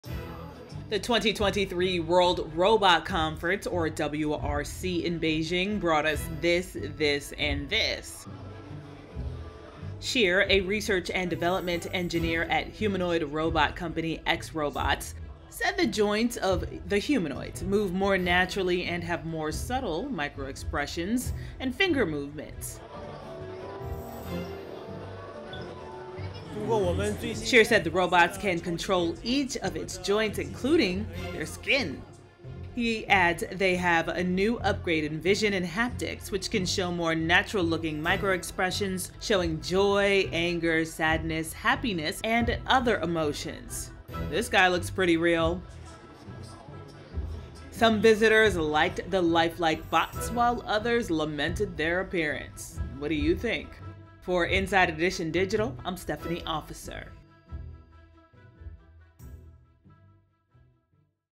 How Would You Like a Humanoid Robot?
Visitors were treated to a display of vivid humanoid robots among others at the 2023 World Robot Conference in Beijing on August 17. "I think they look very real, including their eyes, and some of the movements of the features, as well as the smoothness of the hand movements – they are more realistic than original,” one visitor told the Reuters. Inside Edition Digital’s Stephanie Officer has more.